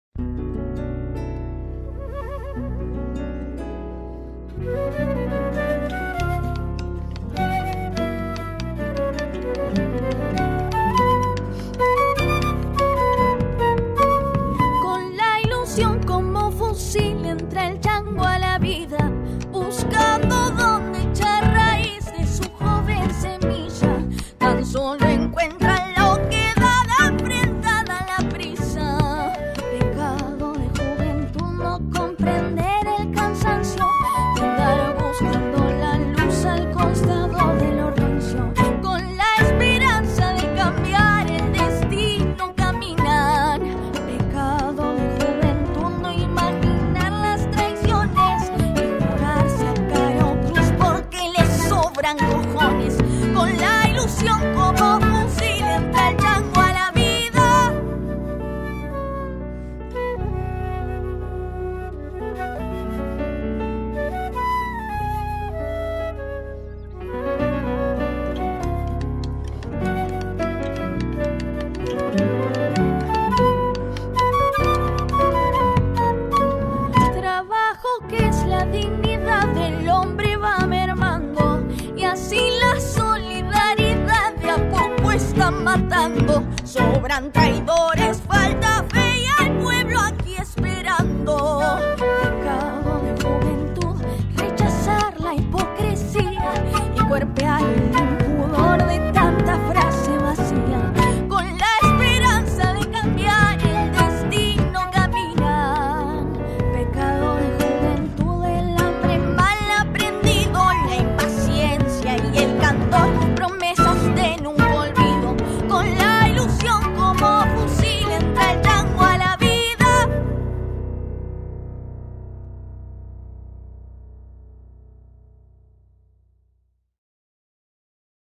Rocío Araujo
Desde el Distrito de Malvinas Argentina, una joven voz se está sintiendo...
Video realizado con motivo de promocionar a esta muchacha  y su visita a los estudios de FM Trujui 90.1 en el Programa GENTE NECESARIA-